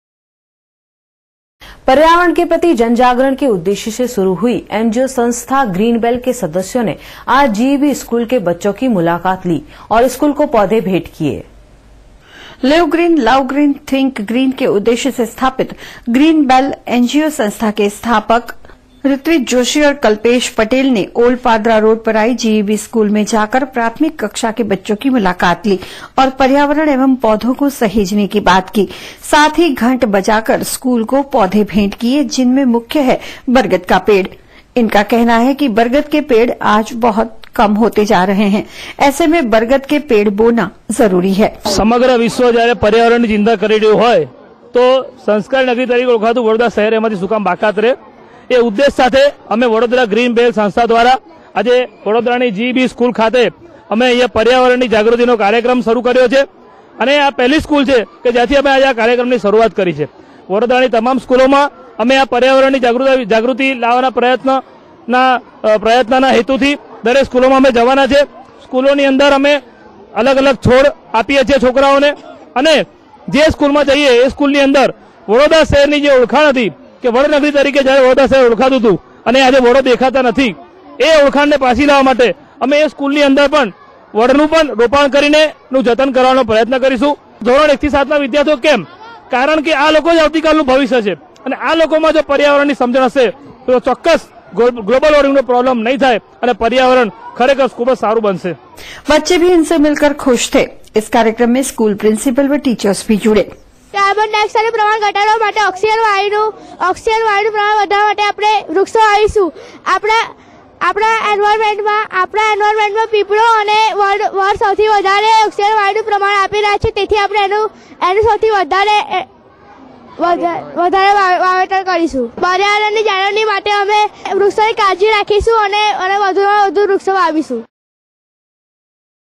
VNM News Vadodara,Gujarat
Much like the torch or the baton for a sports meet, the bell will be taken from one school to another in the city. "We as well as the students of the school where the bell was kept will carry the bell in a specially-made vehicle (GREEN RATH)to the next school,today in GEB School with the students